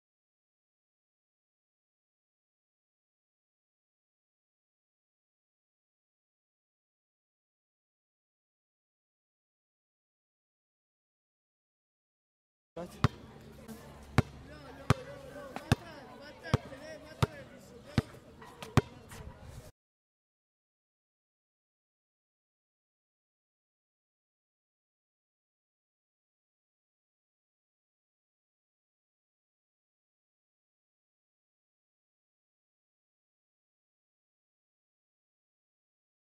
The New Legend - Ep1 - The Start of A Star
A series where we go through the life a boy who wants to be a star